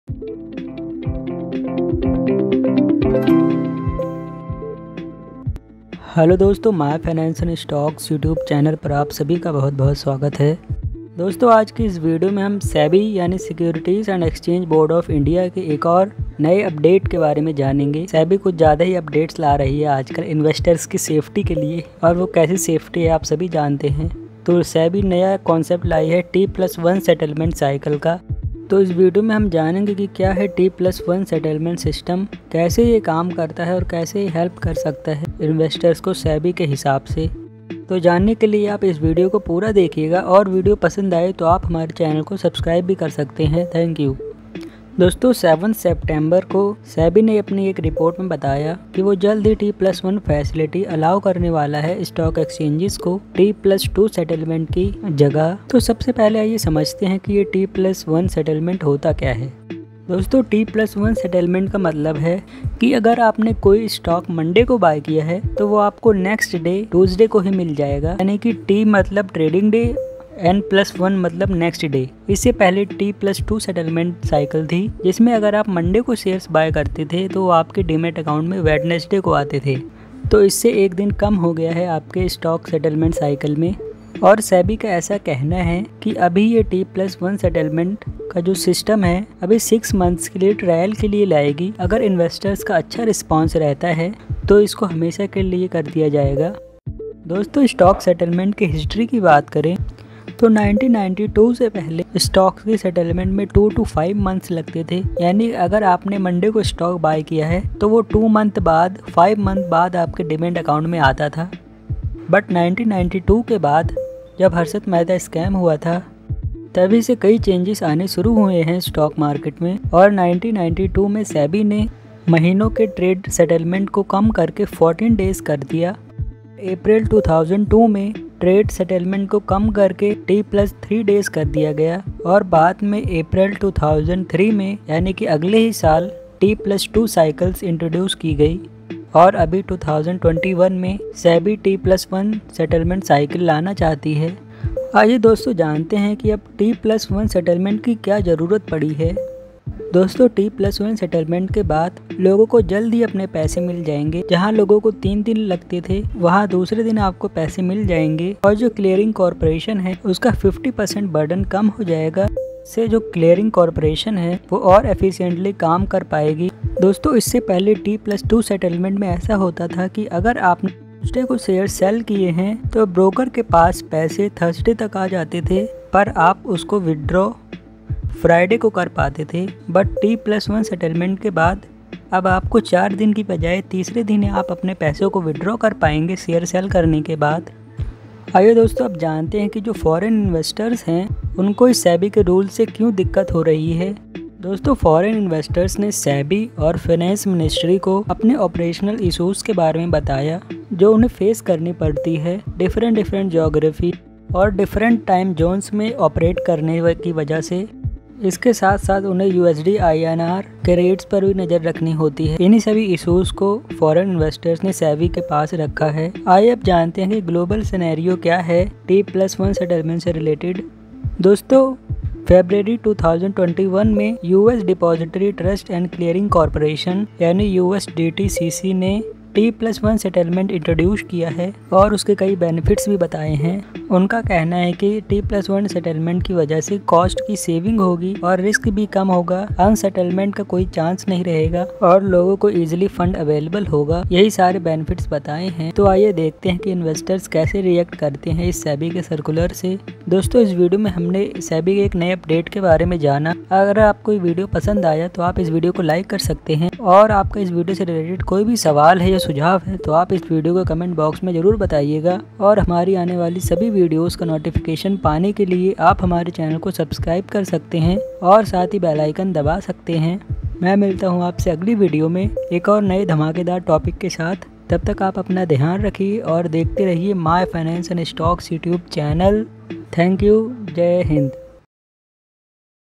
T+1 Settlement Cycle By SEBI | SEBI introduced T+1 Settlement cycle
T+1 Settlement Cycle By SEBI | SEBI introduced T+1 Settlement cycle | SEBI New circular
Hi friends welcome to my finance and stocks youtube channel in this video we will talk about SEBI's new T+1 settlement rule.
Topics covered:
SEBI introduces T+1 Settlement cycle
What is T+1 Settlement cycle
Stocks settlement history
what is the need of T+1 settlement cycle

To Open a Demat Account with India's Leading Stock Brokers,  Click on the link given below:

Stay healthy Stay happy keep watching our videos